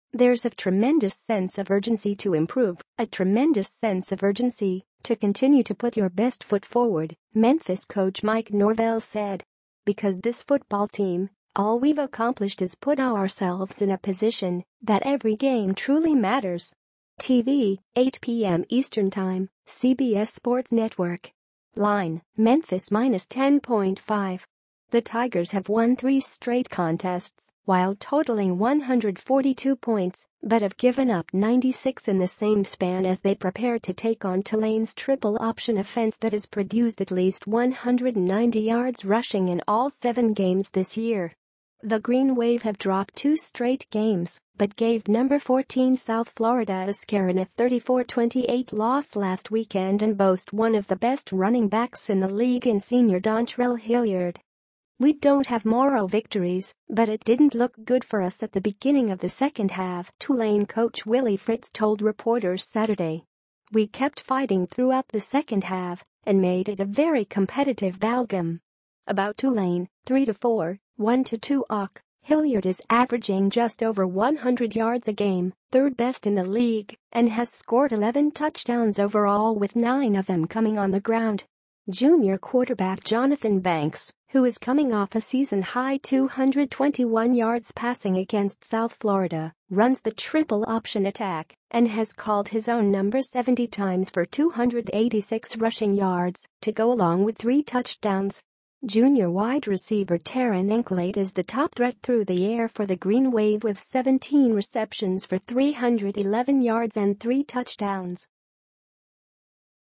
Memphis football vs. Tulane: Time, TV schedule, game preview, score
"There’s a tremendous sense of urgency to improve, a tremendous sense of urgency to continue to put your best foot forward,” Memphis coach Mike Norvell said. "Because this football team, all we’ve accomplished is put ourselves in a position that every game truly matters."

TV: 8 p.m. ET, CBS Sports Network. LINE: Memphis -10.5

The Tigers have won three straight contests while totaling 142 points, but have given up 96 in the same span as they prepare to take on Tulane’s triple-option offense that has produced at least 190 yards rushing in all seven games this year.

The Green Wave have dropped two straight games, but gave No. 14 South Florida a scare in a 34-28 loss last weekend and boast one of the best running backs in the league in senior Dontrell Hilliard.

“We don’t have moral victories, but it didn’t look good for us at the beginning of the second half,” Tulane coach Willie Fritz told reporters Saturday. “We kept fighting throughout the second half and made it a very competitive ballgame.”

ABOUT TULANE (3-4, 1-2 AAC)
Hilliard is averaging just over 100 yards a game - third best in the league - and has scored 11 touchdowns overall with nine of them coming on the ground. Junior quarterback Jonathan Banks, who is coming off a season-high 221 yards passing against South Florida, runs the triple-option attack and has called his own number 70 times for 286 rushing yards to go along with three touchdowns. Junior wide receiver Terren Encalade is the top threat through the air